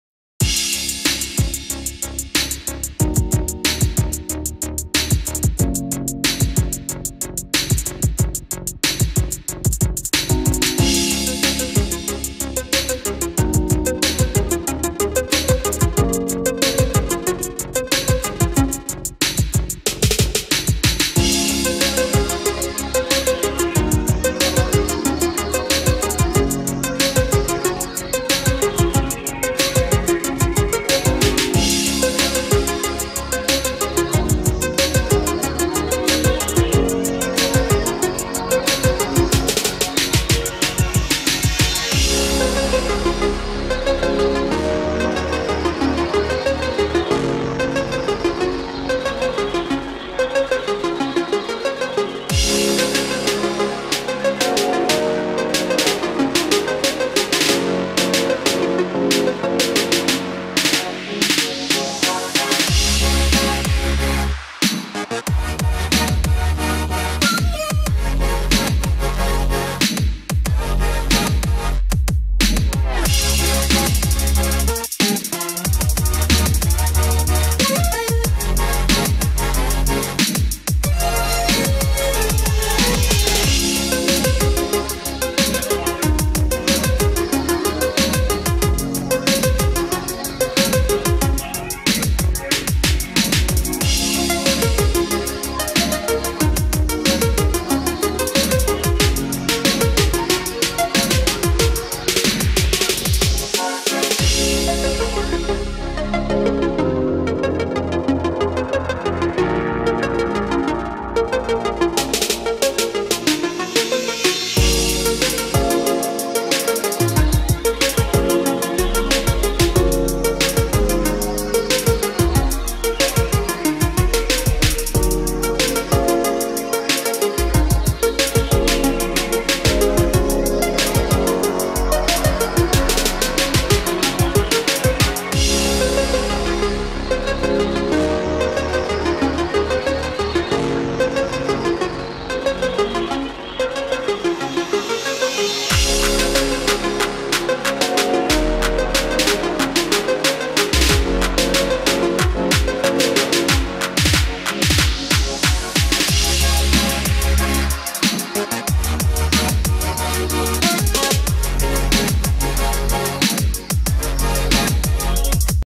Malabars a la Plaça Virreina 2011
Malabars a la Plaça Virreina al 2011.
3 maces, 4 i 5 pilotes.